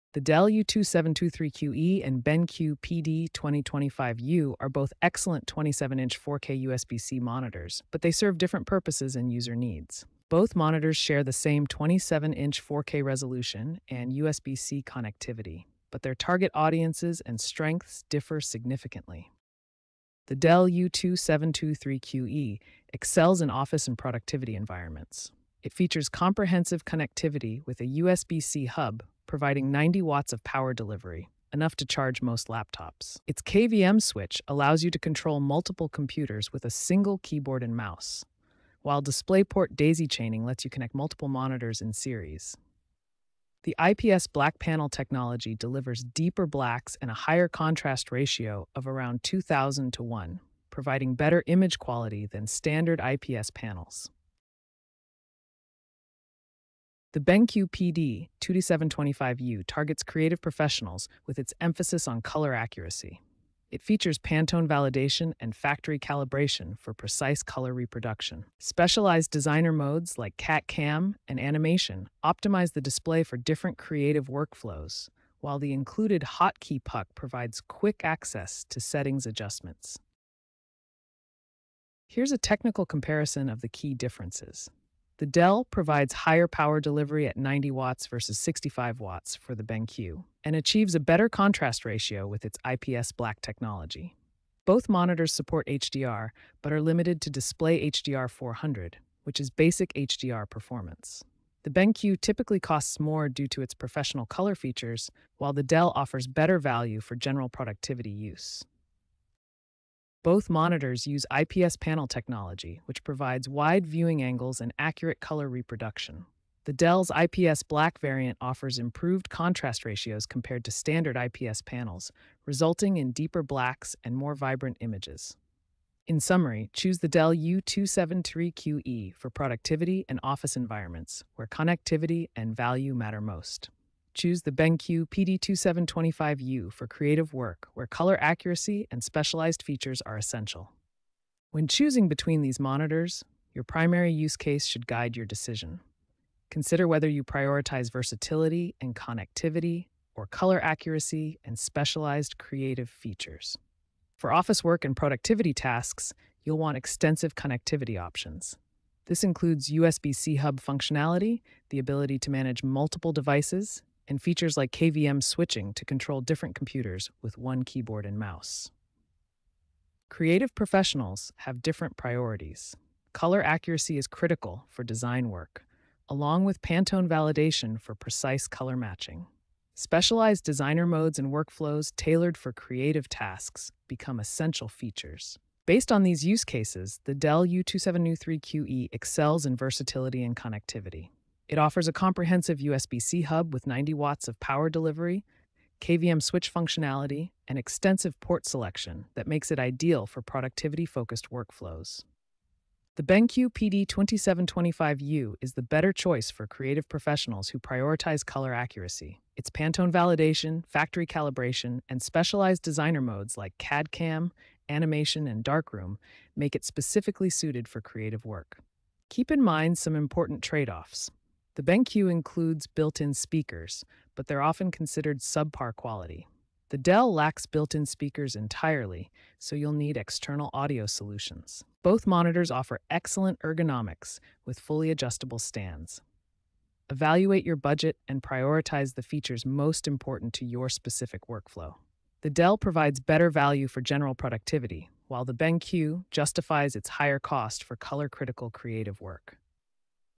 Dell U2723QE Vs BenQ PD2725U – 4K USB-C Monitor Comparison
Discover the key differences between the Dell U2723QE and BenQ PD2725U in our detailed 4K USB-C monitor comparison. Find the perfect display for your needs.